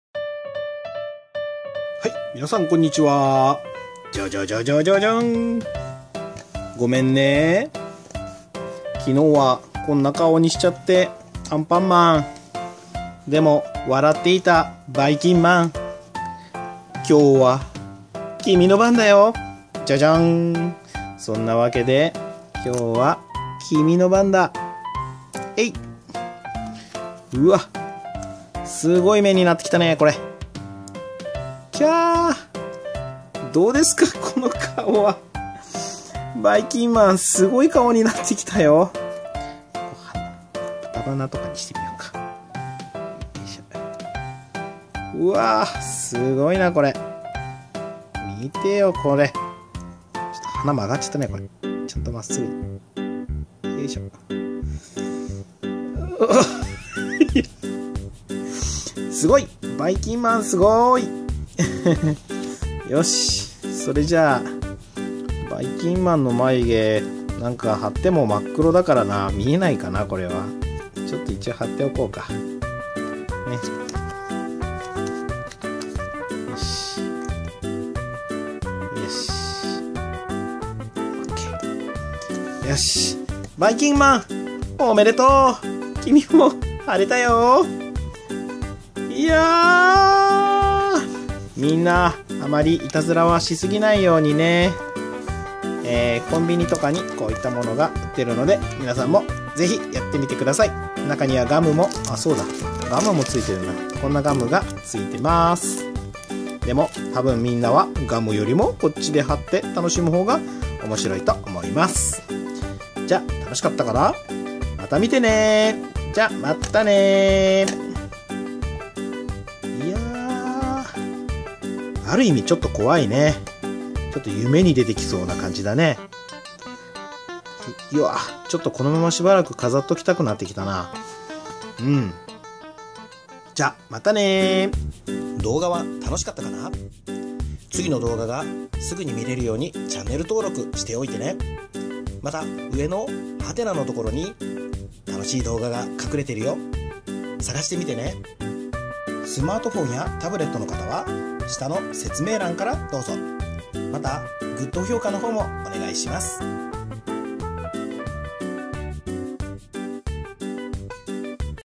アンパンマン❤おもちゃ 変顔対決アンパンマンVSバイキンマン！
今日は「アンパンマン❤おもちゃ 変顔対決アンパンマンVSバイキンマン！」だよ！

 @キッズ アニメ&おもちゃ

BGM、効果音につきましては、概要欄に記載させて頂きました。
使用させて頂き、ありがとうございました。